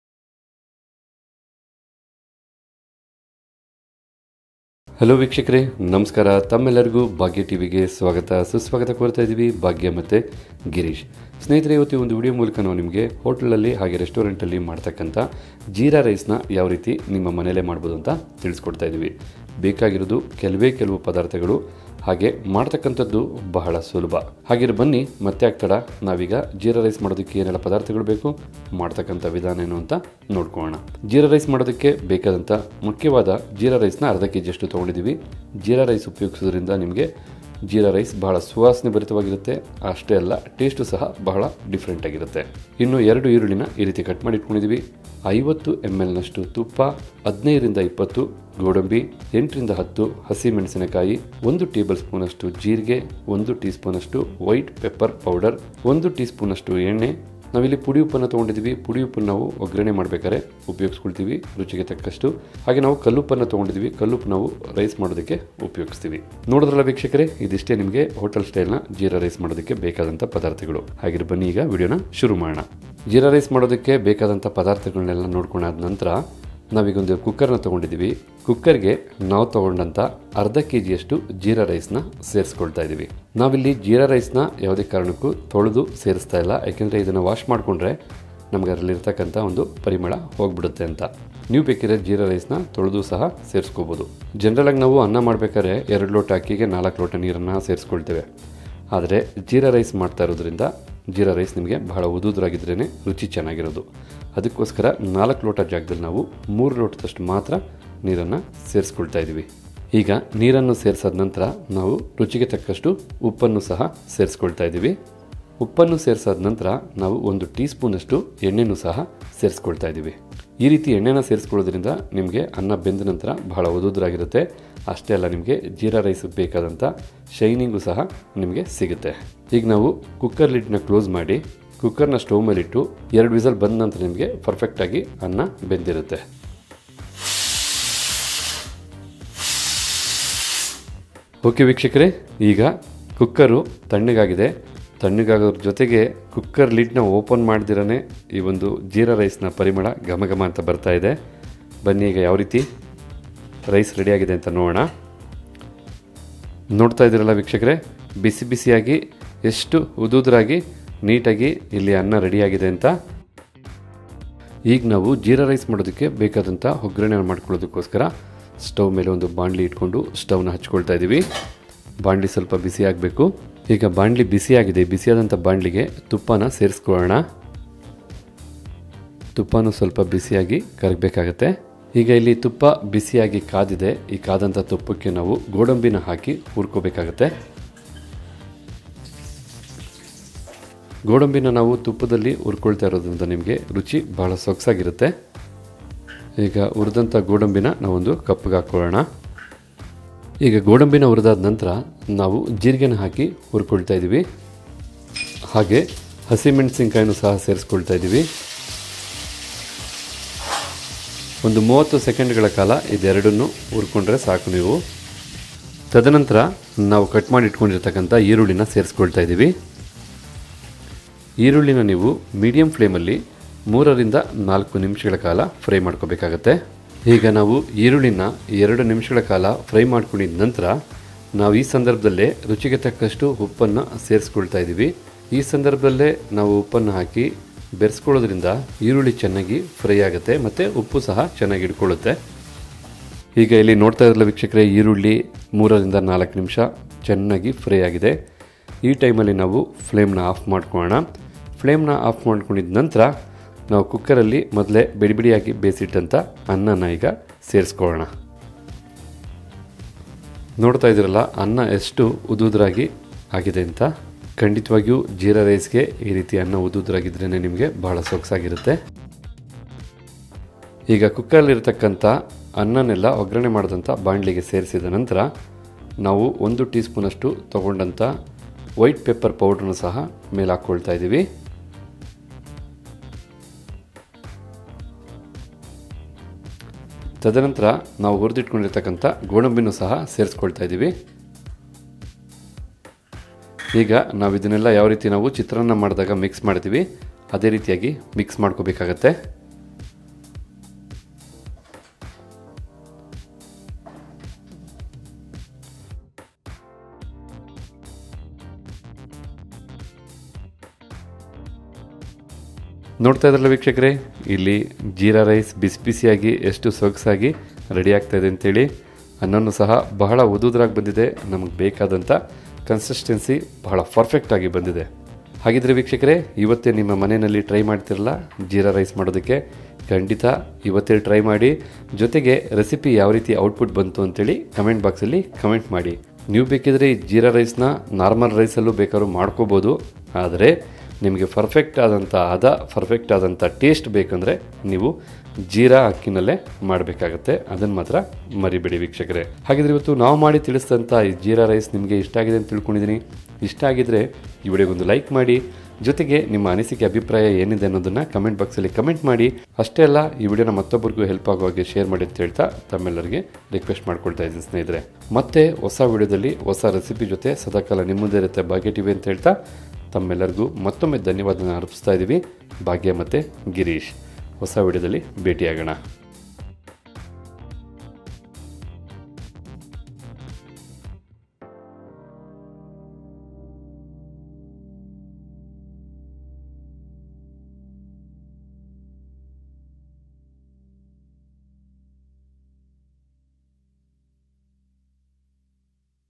ಜೀರಾ ರೈಸ್ 100% ಹೋಟೆಲ್ ಸ್ಟೈಲ್ನಲ್ಲಿ | Jeera Rice recipe in kannada | How to Make Perfect Jeera Rice
Required ingredients for jeera rice :
Jeera rice raw rice 500 gm
White pepper powder 1 tbsp
Cashew nut 15 to 20